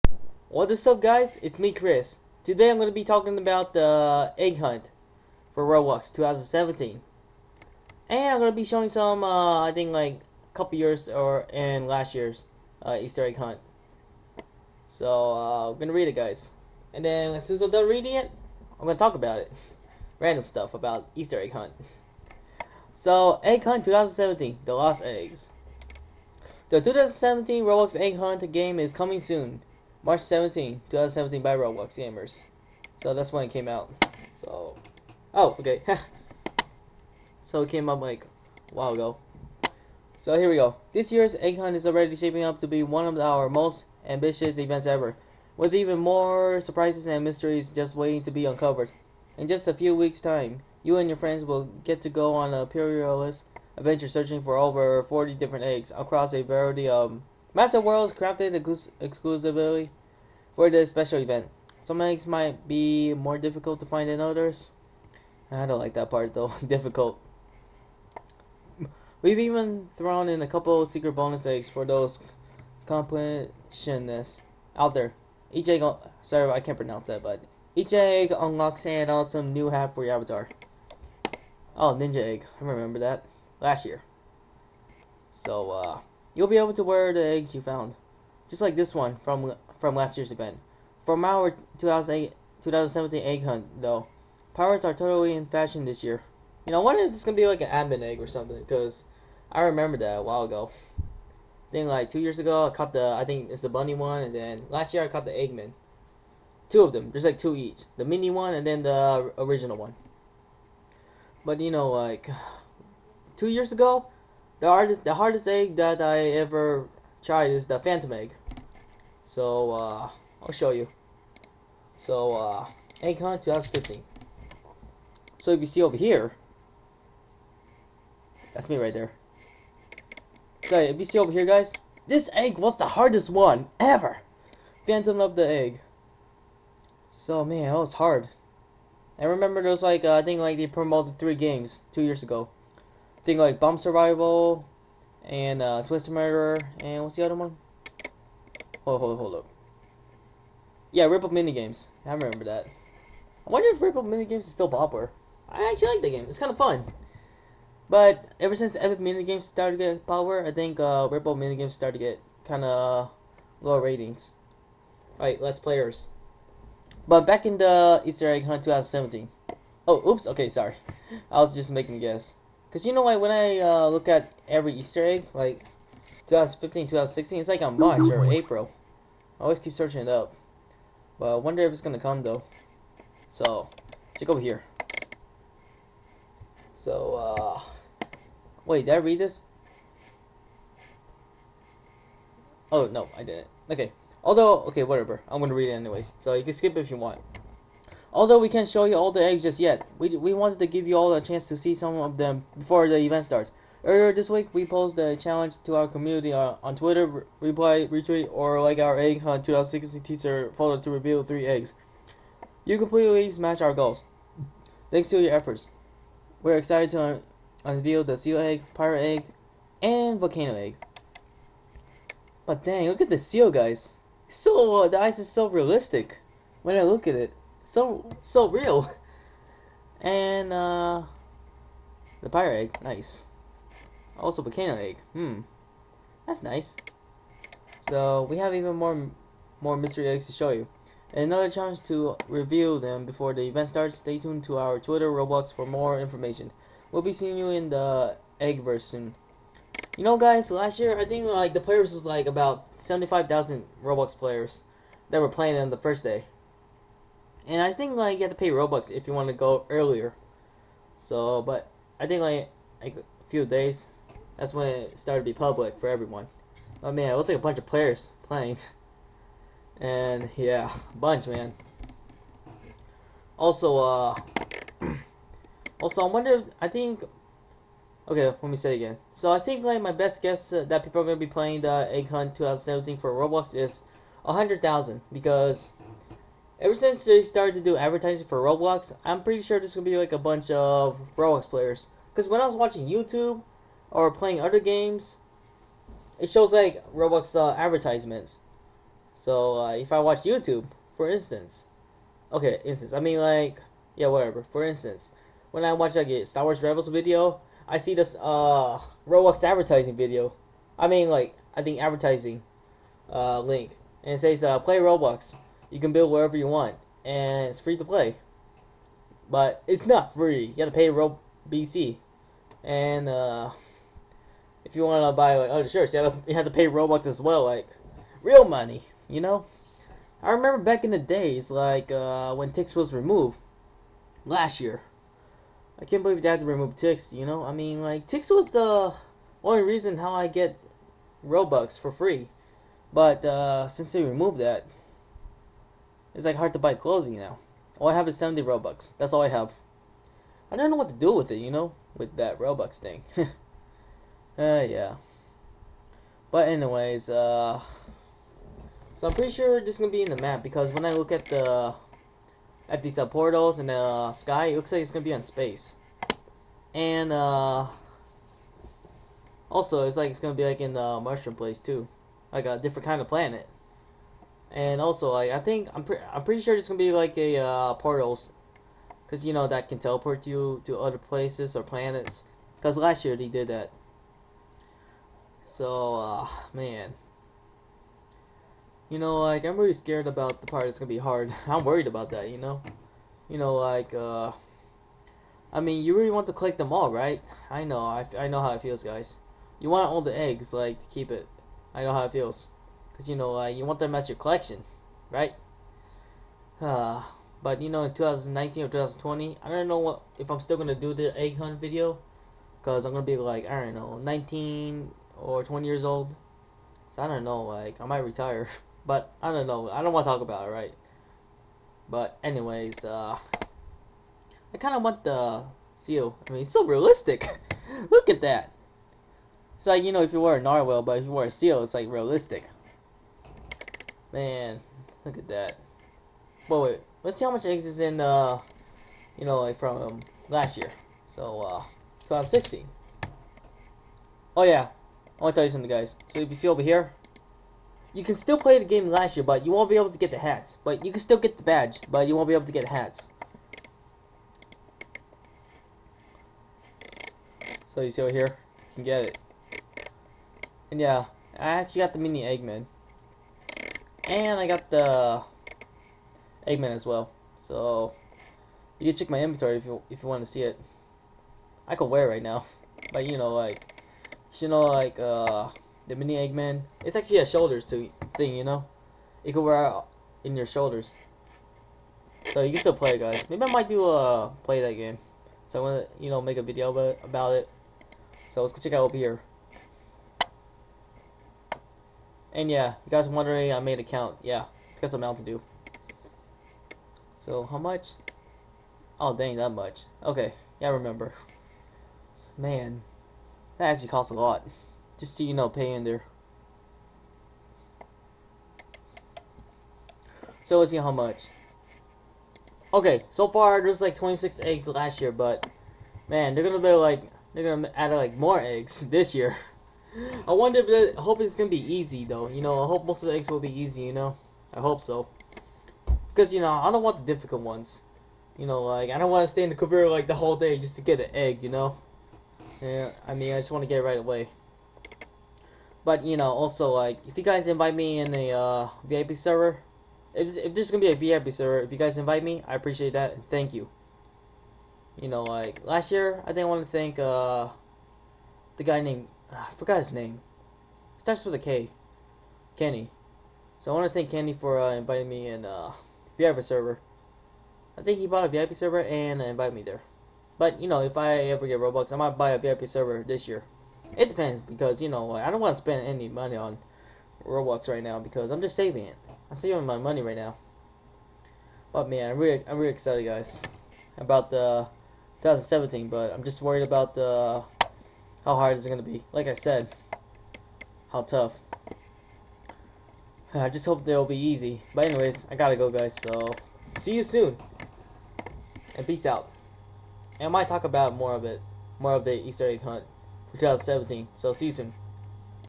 Talking About the Roblox Easter Egg Hunt 2017
There are more eggs compare to last year. There were 26 eggs last year, and now they've added more this year. I hope it'll be easy, I sometimes don't like the difficult once. I hope it'll be a good year too just like last year in 2016.